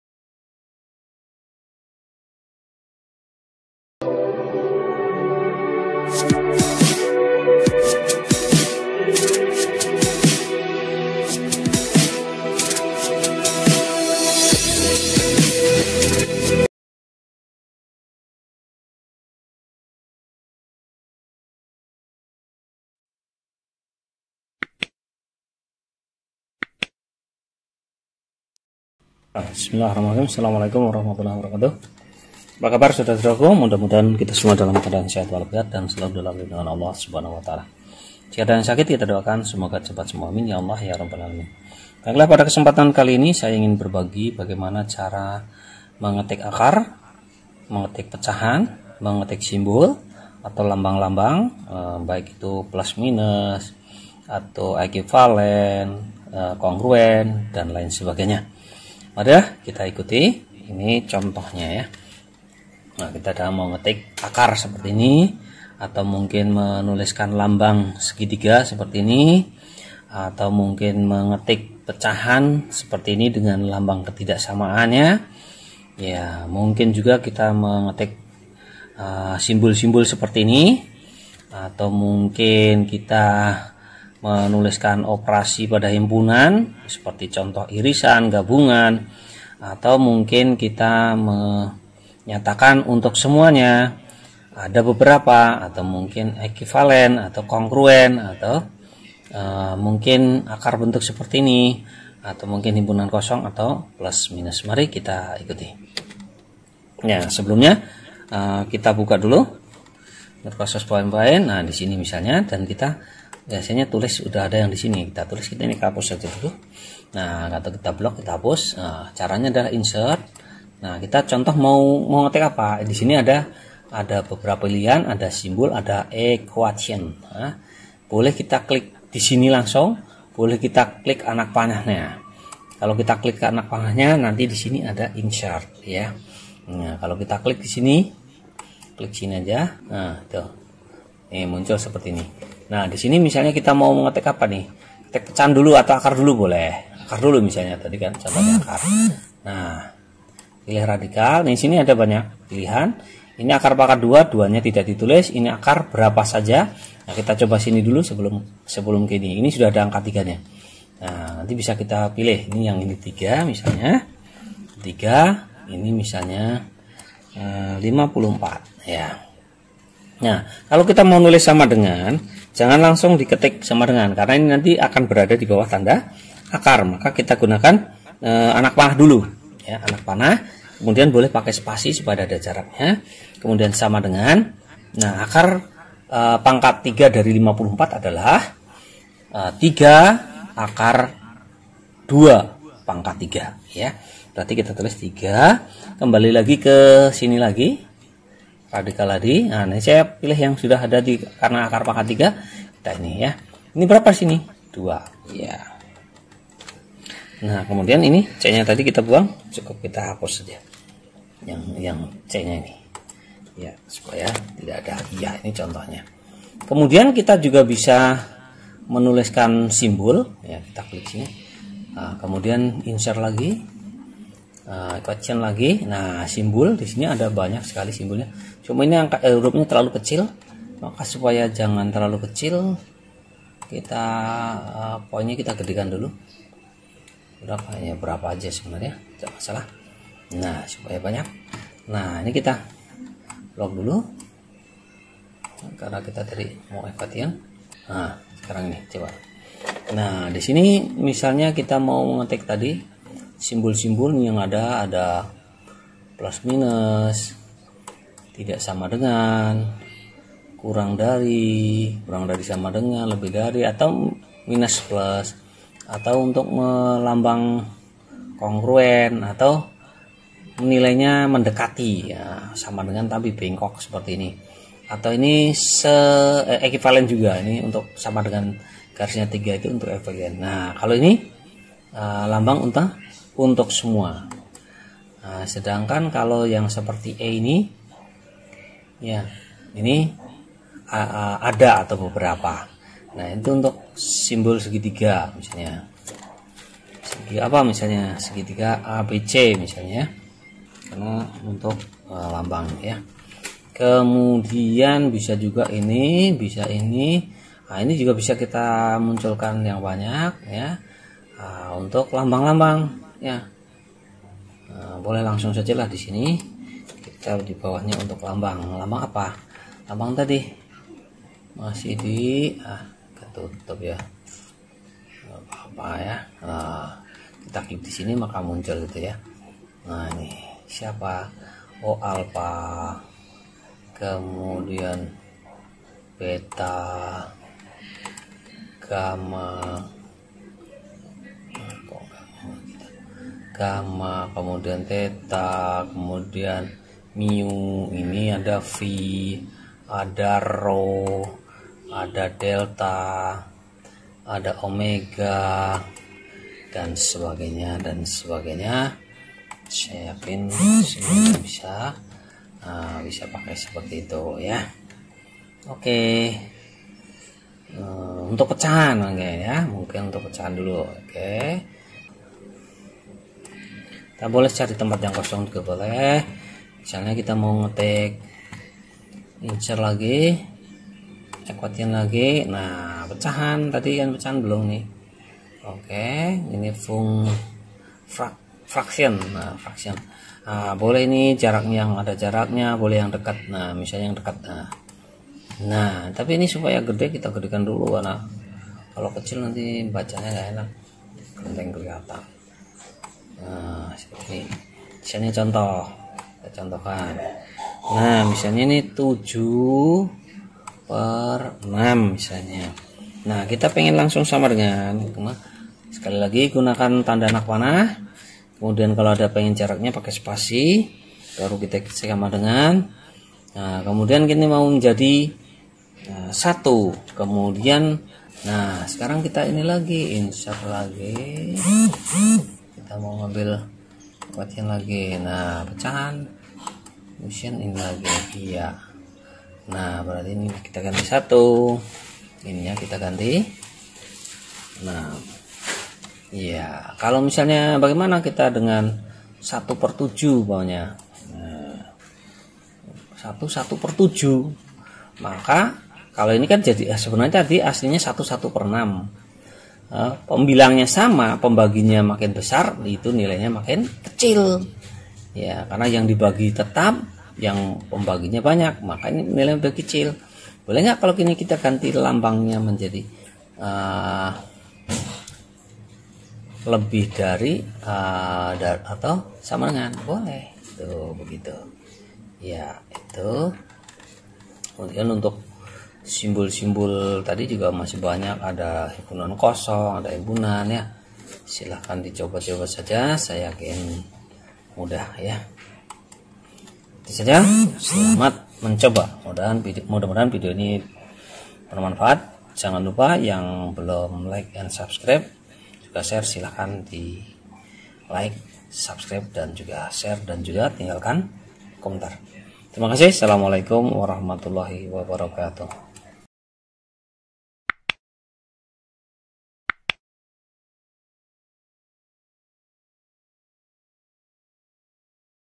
Tutorial Cara Mudah Ketik Akar Pecahan Simbul dan Lain-lain Pada Microsoft PowerPoint
Video ini berisi tentang Tutorial Cara Mudah Ketik Akar Pecahan Simbul dan Lain-lain Pada Microsoft PowerPoint
Hal kecil yang sepele namun kadang membuat kita bingung kalua tidak tahu caranya contohnya adalah mengetik Akar, Pecahan, Simbul atau lambang lainnya pada waktu kita kerja dengan Microsoft Power Point
Ikuti Cara mudahnya sesuai yang ada pada video ini